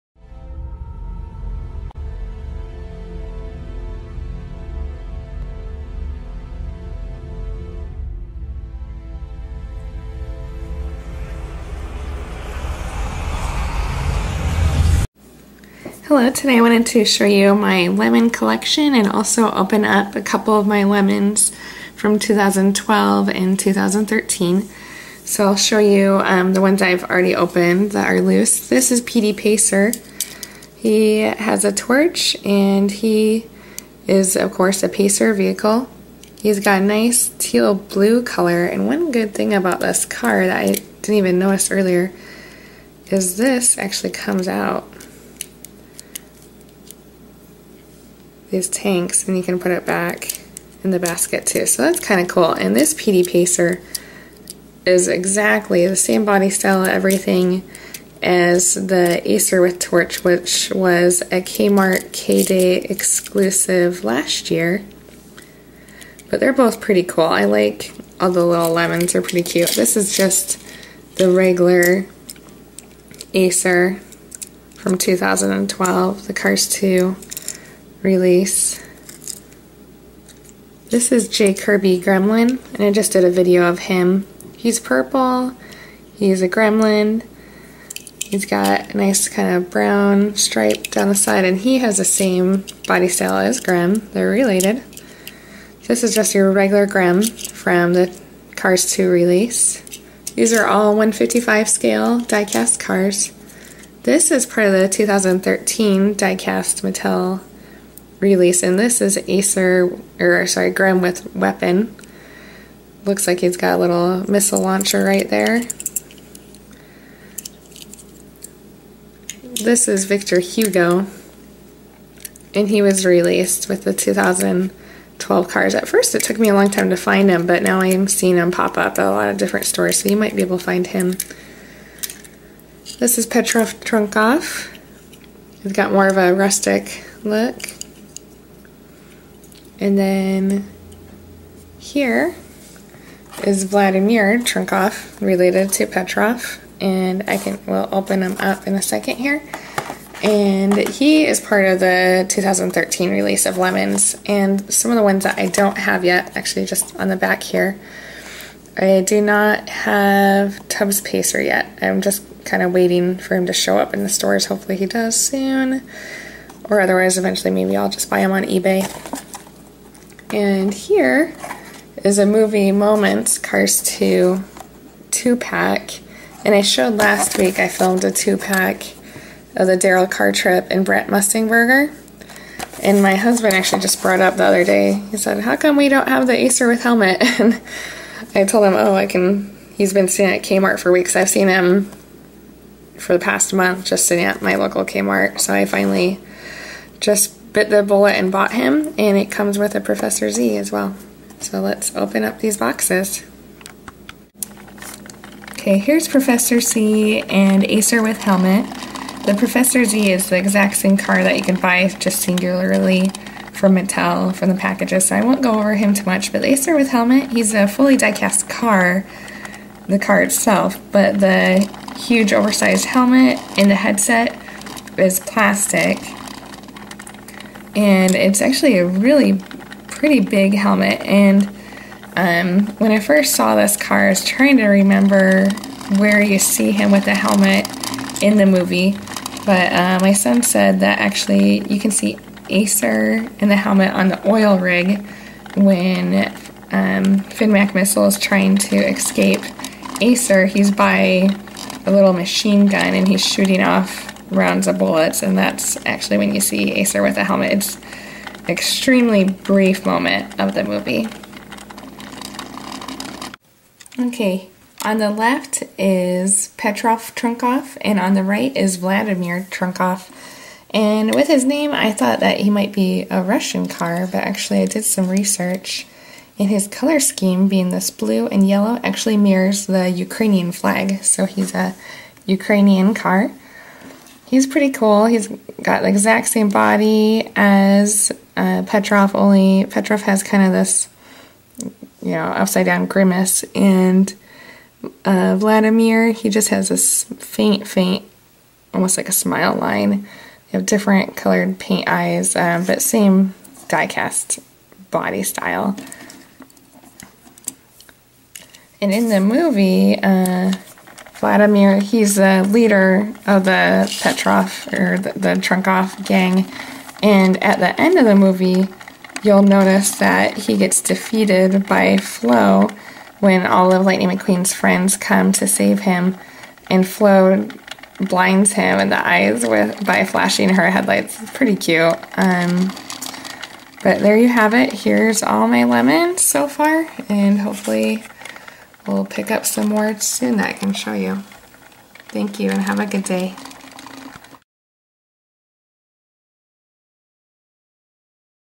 Cars 2 Bad Guys Lemons Acer with Helmet, Vladimir Trunkov, Petey Pacer, J Curby Gremlin
Complete collection of Disney Pixar Cars 2 Lemons (bad guys). This video includes my bad guys collection of Acer and Acer with Torch, Acer with Helmet.  It also has Grem and Grem with Weapon with Petey Pacer with Torch and more!  All the cars in this video are 1:55 scale diecast.  This video was made by the "DisneyCarToys" channel.